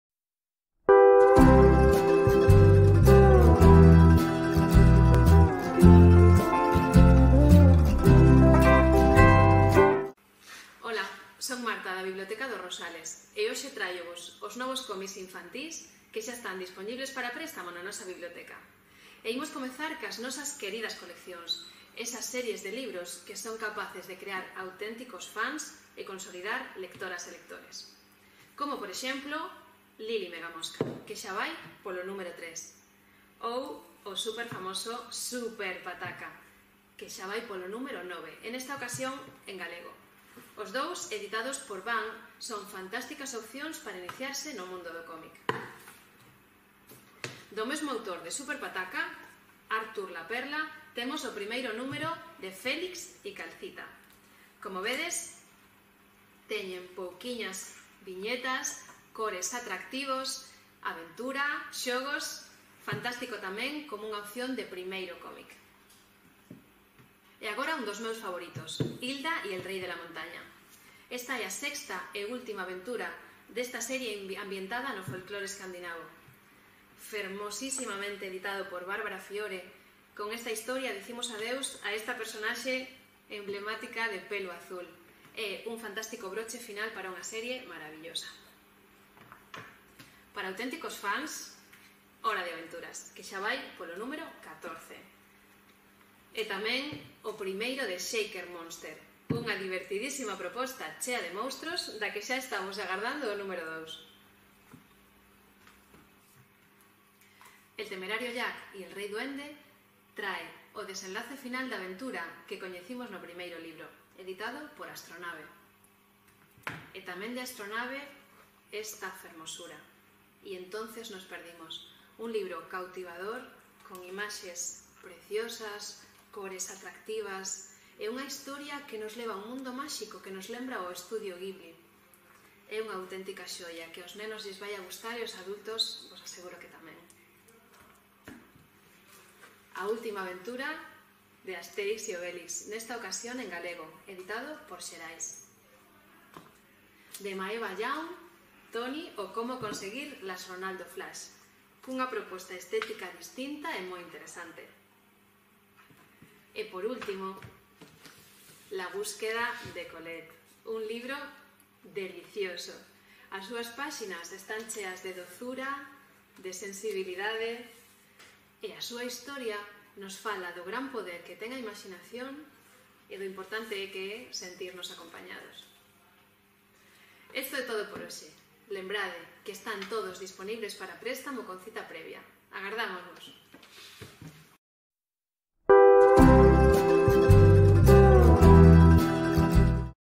Novidades do comic infantil na Btca. Municipal
Os últimos cómics adquiridos nas Btca. Municipal Rosales para nenos e nenas. Xuño de 2020